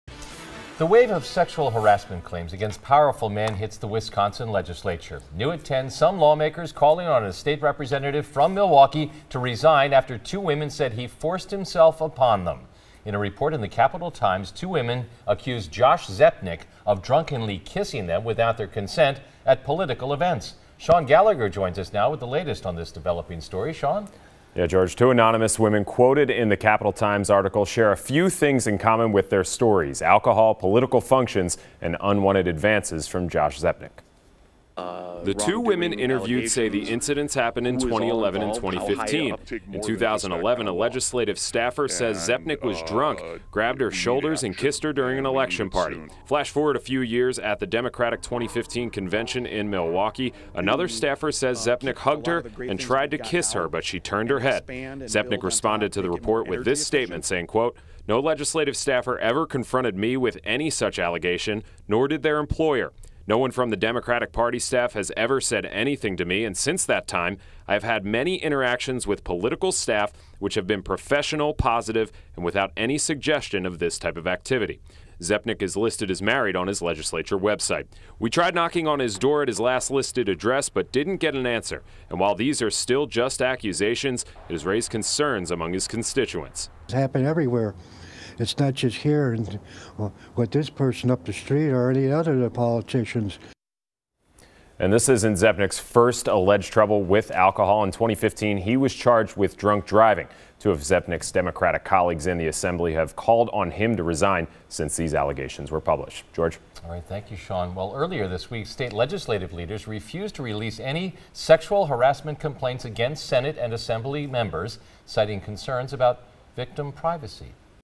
Report: Women accuse Milwaukee legislator of sexual misconduct
A newspaper is reporting that two women have accused Democratic state Rep. Josh Zepnick of sexual misconduct.